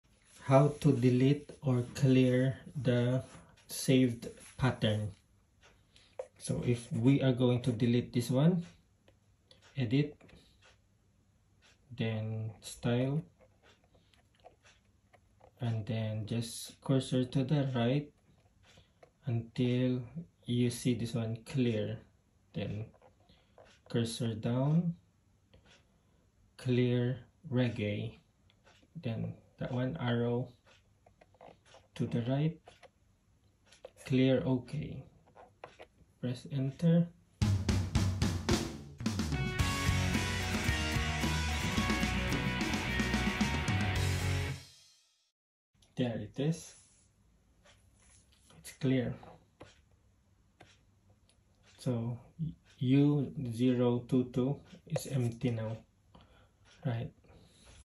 How to delete or clear the saved drum pattern from Boss Dr Rhythm DR3 drum machine
Boss Dr Rhythm DR3 drum machine Delete or Clear drum pattern that you created or saved.